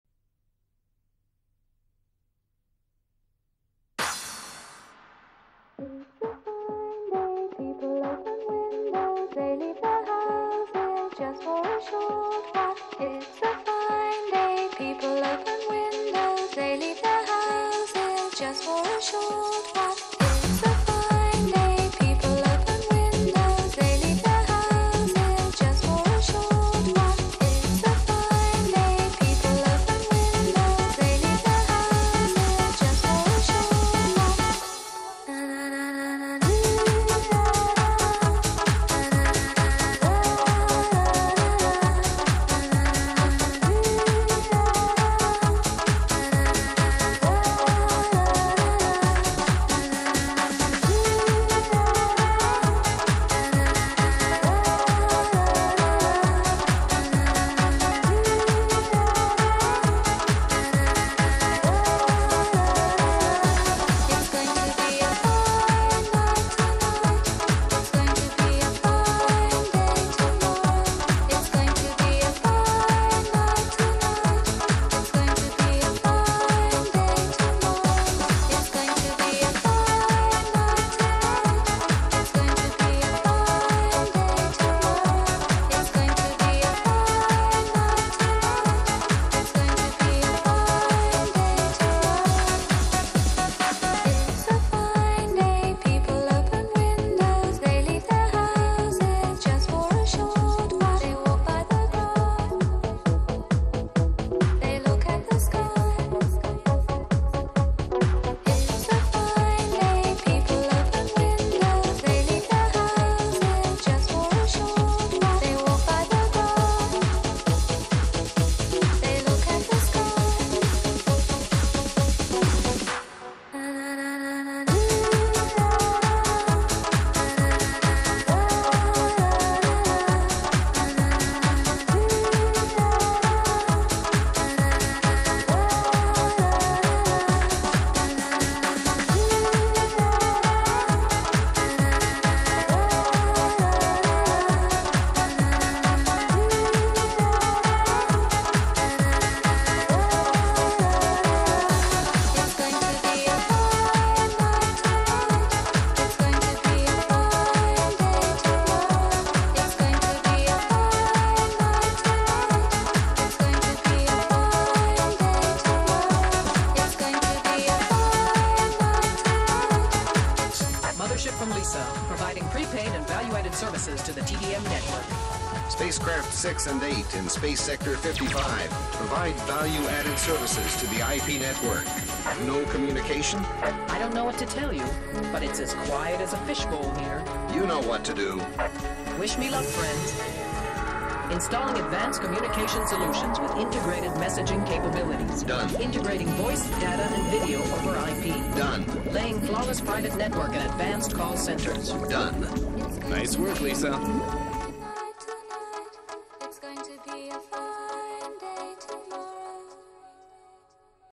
דוכן טלרד בתערוכת Ce-Bit 2000
דוכן חברת טלרד היה להצלחה גדולה בתערוכת Ce-Bit בשנת 2000.